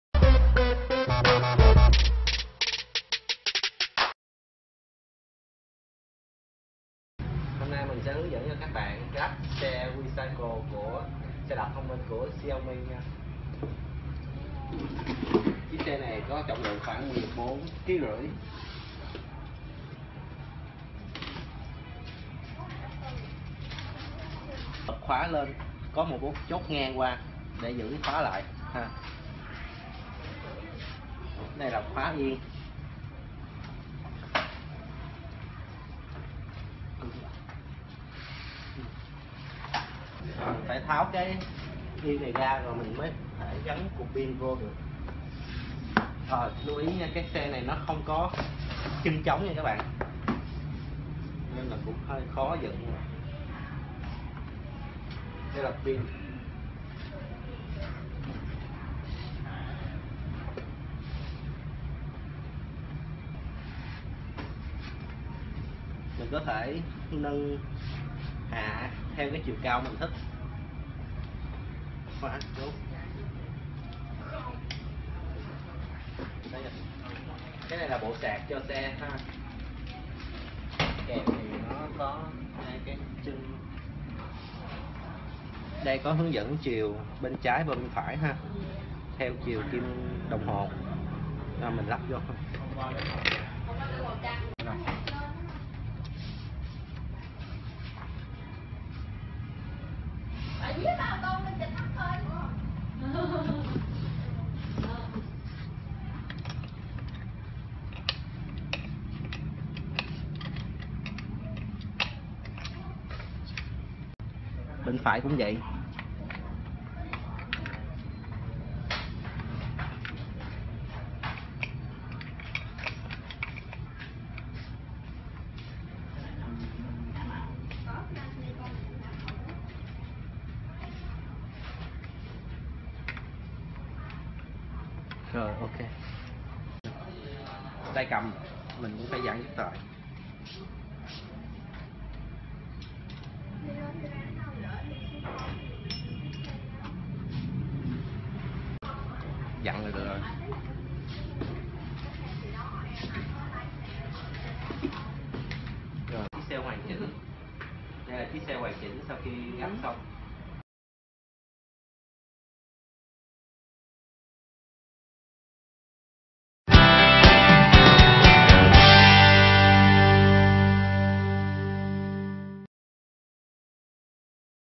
Hướng dẫn lắp ráp xe đạp điện thông minh QiCycle Xiaomi - [ Chiếm Tài Mobile ]
Xe đạp điện thông minh QiCycle Xiaomi
Mới đây nhất hãng Appble của Trung Quốc Xiaomi đã cho ra mắt sản phẩm mới nhất của mình. Đó chính là chiếc Xe đạp điện thông minh QiCycle với những tính năng chuyên dụng có khả năng hỗ trợ người sử dụng một cách tối ưu nhất.
Với việc cho ra đời dòng sản phẩm mới xe đạp điện thông minh QiCycle này thì Xiaomi là một lần nửa chứng minh cho thế giới rằng Xaiomi không chỉ là một công ty sản xuất điện thoại thông minh Smartphone mà Xiaomi thật sự là một công ty về công nghệ. Khả năng định hướng và mở rộng của công ty Xiaomi sẽ còn đi xa hơn những gì mà thế giới nhìn nhận về họ hiện nay.

Hướng dẫn lắp ráp xe đạp điện thông minh  QiCycle Xiaomi
Nên mua xe đạp thông minh xiaomi ở đâu
Có nên mua xe đạp thông minh xiaomi không ?